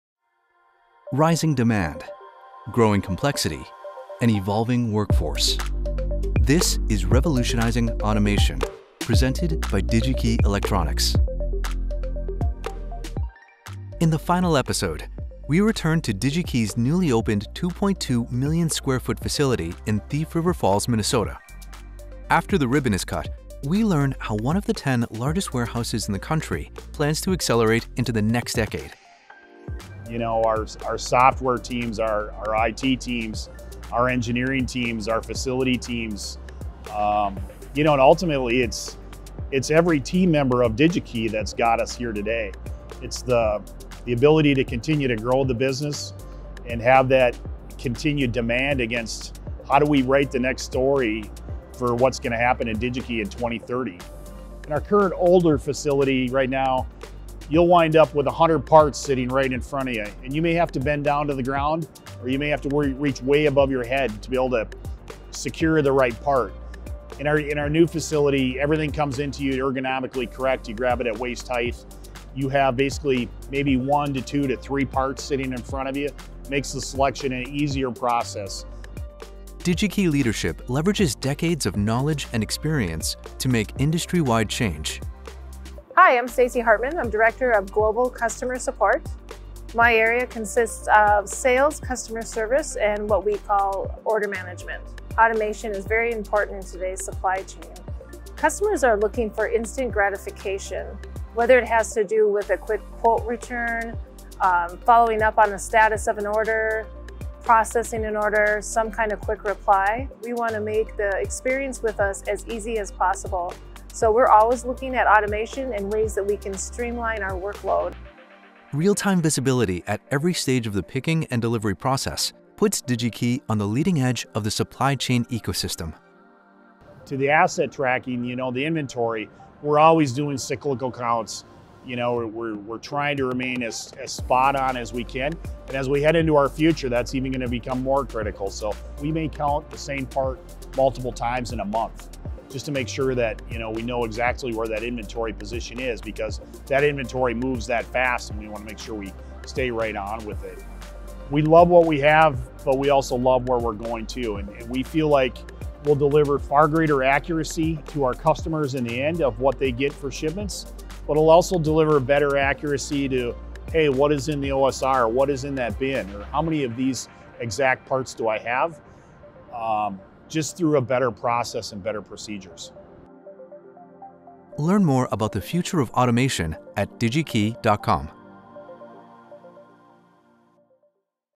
Revolutionizing Automation - Future of Automation at Digi-Key | Digi-Key Electronics
In the final episode, we visit Digi-Key's newly opened, 2.2 million-square-foot, PDCe in Thief River Falls, Minnesota. After the ribbon is cut we learn how one of the 10 largest warehouses in the country plans to accelerate into the next decade.

We sit down with Digi-Key leadership, including Vice President, Order Fulfillment at Digi-Key, Chris Lauer, to talk about new areas of growth and the implementation of technology that pushes the envelope in regards to automation.

What are the challenges shipping 13.4 million products to more than 857,000 customers each year in 180 countries?

How is Digi-Key leveraging new forms of technology within this new distribution warehouse?

What does the future of automation look like at Digi-Key?